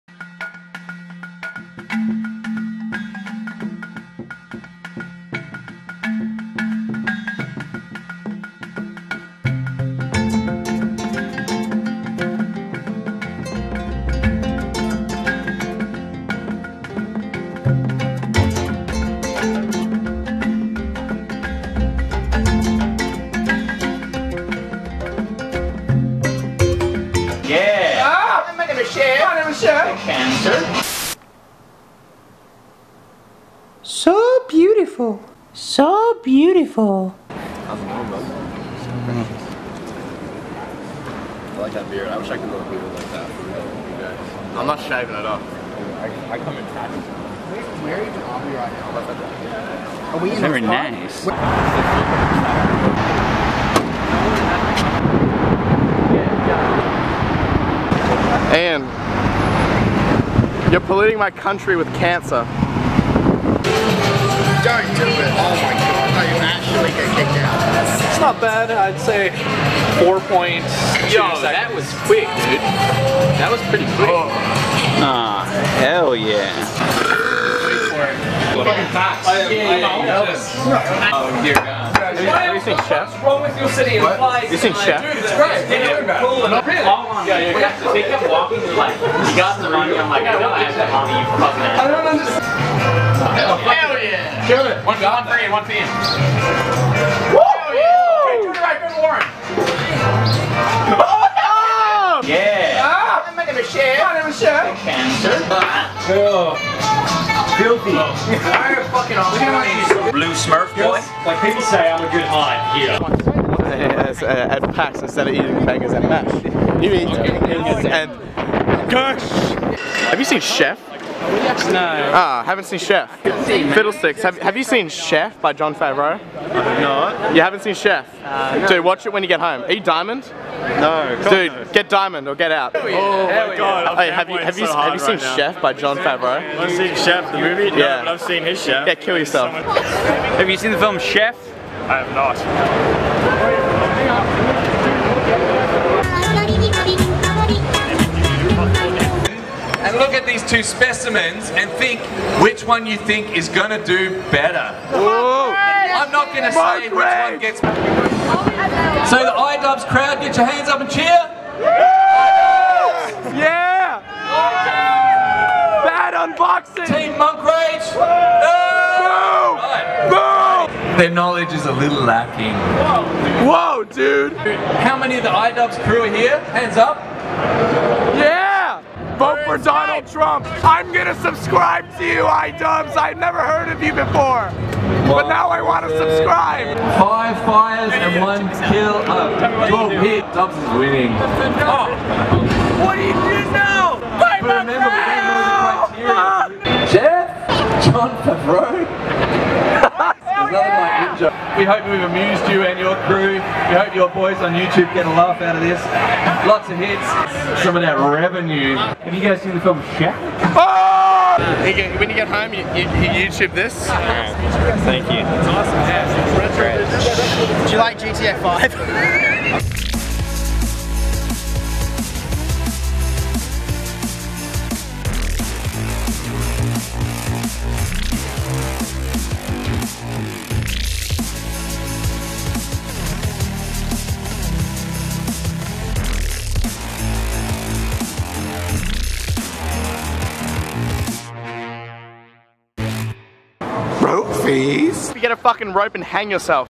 PAX Australia 2015
Here a little bit of my trip to Australia. Sorry if you saw most of it on max's vlog. Watch the very end, after the outro, anything4views tells a little kid to kill himself. hilarious stuff.

Remember to hit the "like" button, if you enjoyed it. It helps my ego!